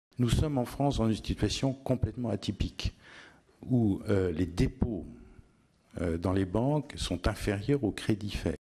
[Loi Bancaire] La situation "atypique" de la France
Audition à l'Assemblée Nationale le 30 janvier 2013, dans le cadre de la loi de (non-) séparation des activités bancaires.

Intervention erronée du gouverneur de la banque de France (et ancien directeur de cabinet d'Edouard Balladur en 1986).